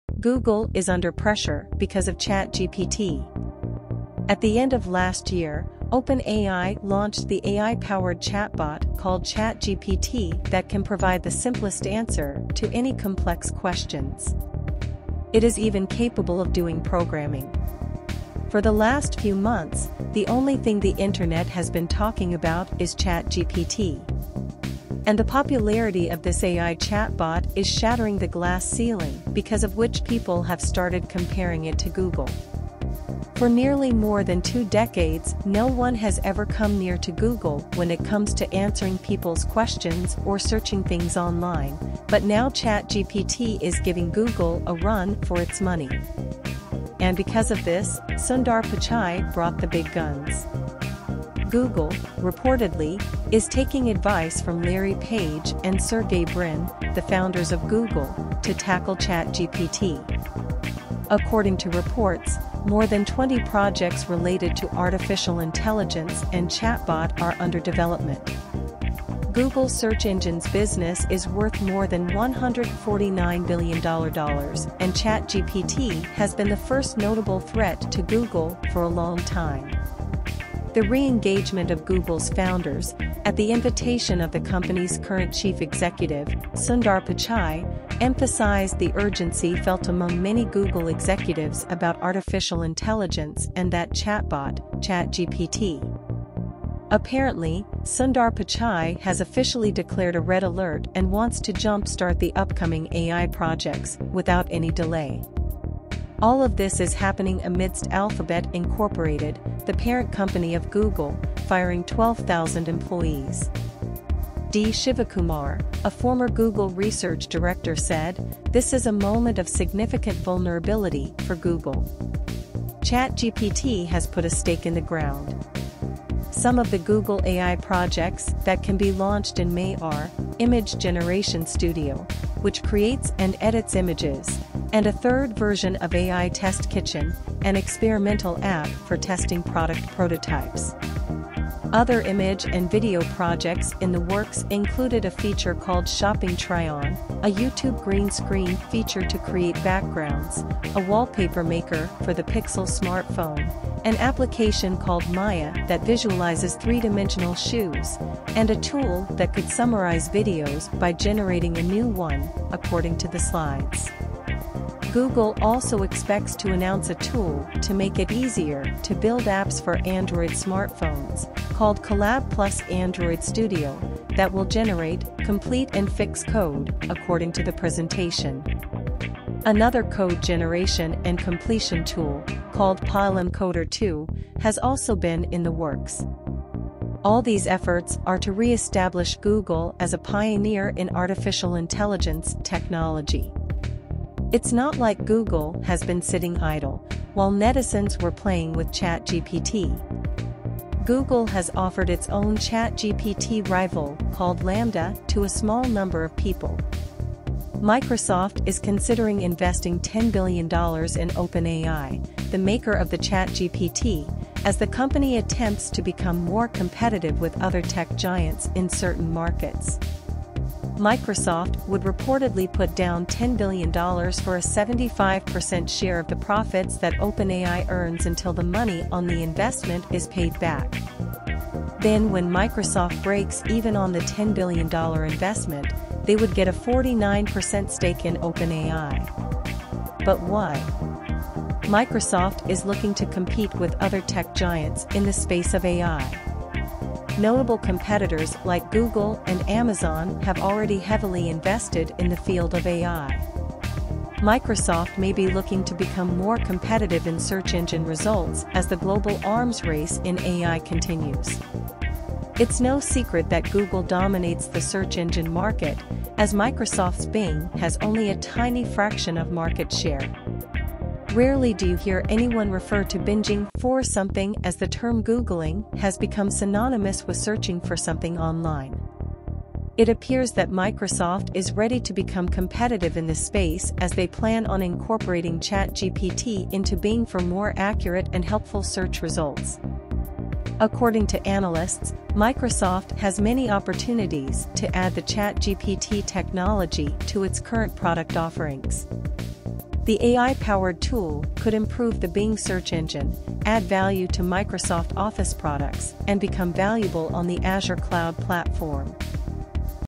Google reacts to ChatGPT | Sundar Pichai asking help from Larry Page | Microsoft invests $10 Billion
Sundar Pichai says Google is on "Red Alert" and wants to move things in motion as quickly as possible. For this, he brings back the founders of Google, Larry Page, and Sergey Brin, on the team. Microsoft investing $10 Billion in ChatGPT and empowering Bing is worrying Google.

For this, Google is ready to launch nearly 20 artificial intelligence products in May that will focus on AI chatbots and content creation.